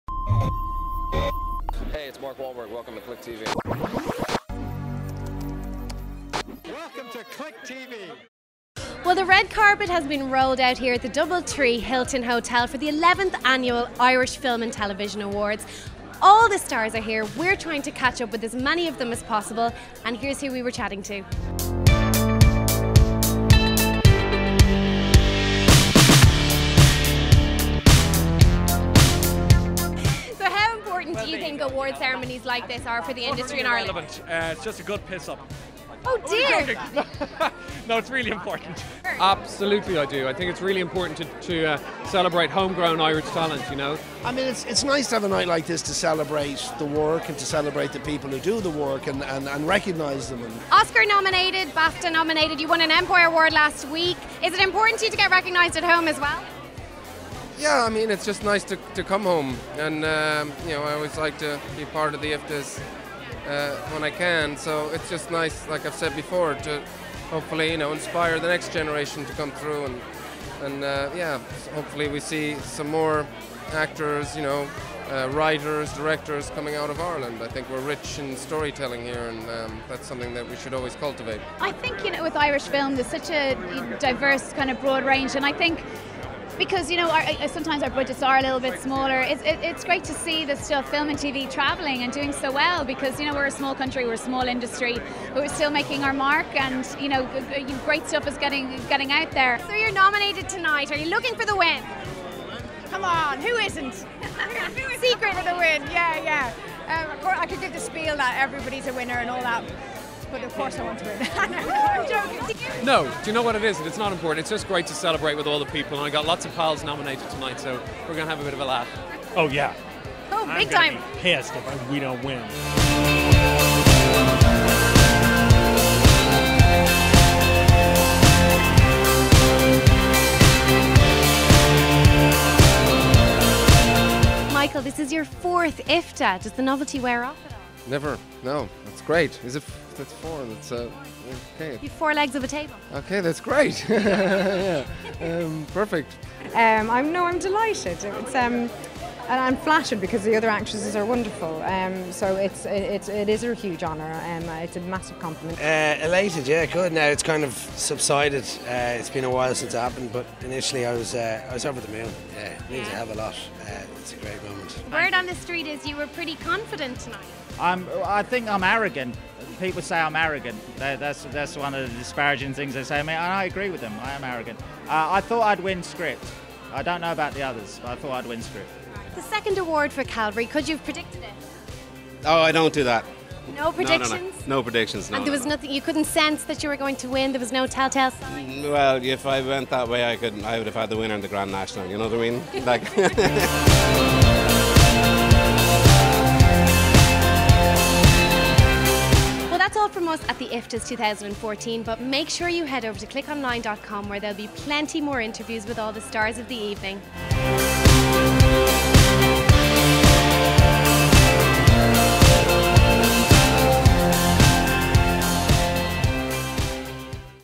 IFTA Red Carpet Interviews - Brendan Gleeson, Michael Fassbender, Will Forte
Click went along to the Irish Film and TV awards in Dublin to catch up with all the stars, including Michael Fassbender, Brendan Gleeson, Andrew Scott, Will Forte, Colm Meaney, Amy Huberman and more.